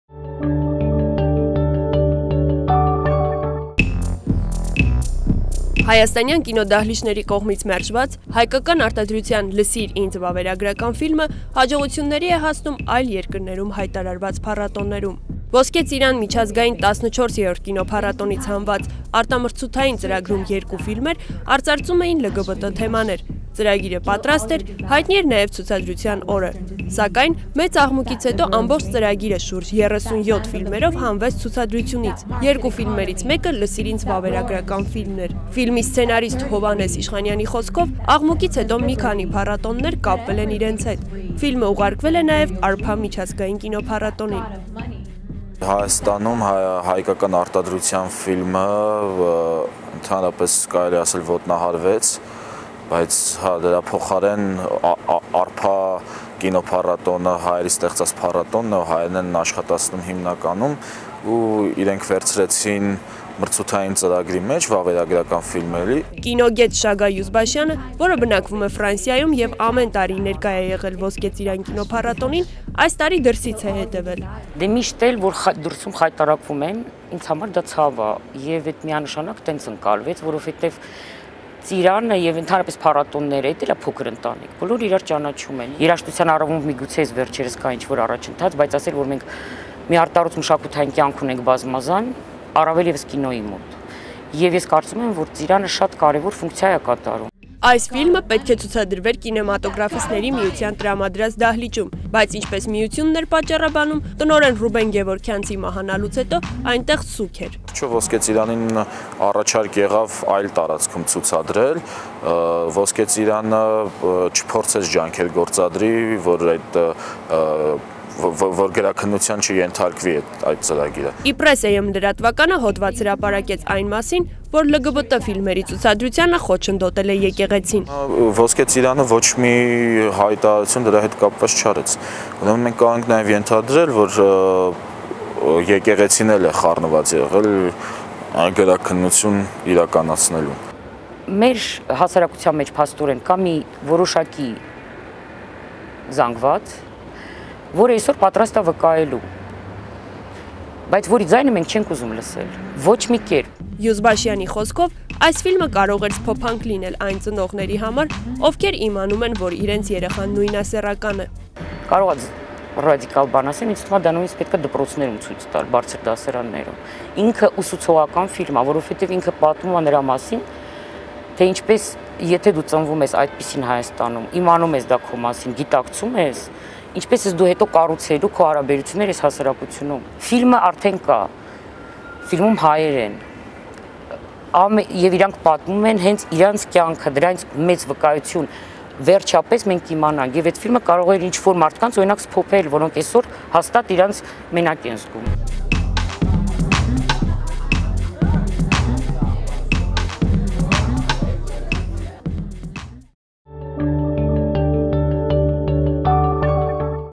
«Լսիր ինձ»․ Ոսկե ծիրանում մերժված ֆիլմը ցուցադրվում է ԱՄՆ-ում
«Լսիր ինձ» վավերագրական ֆիլմն ընդգրկվել է «Արփա» միազգային կինոփառատոնի մրցութային ծրագրում: ԼԳԲՏ թեման արծարծող այս ֆիլմը «Ոսկե ծիրան» միջազգային 14-րդ կինոփառատոնի արտամրցութային ծրագրից հանված 37 ֆիլմերից մեկն էր։